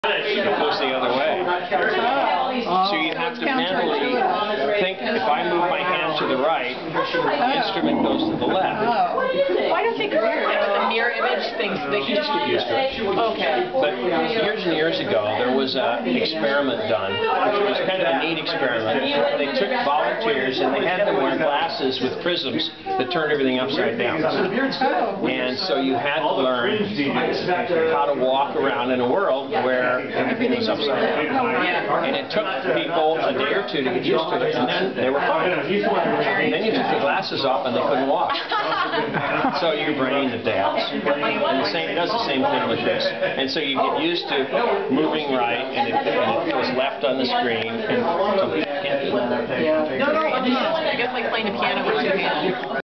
Mike K. explains it
Dr. Mike Kennedy explains how surgeons get used to seeing reversed images, using as an example a perceptual experiment. 18 in the palatial digs of critic Ray Richmond in Studio City.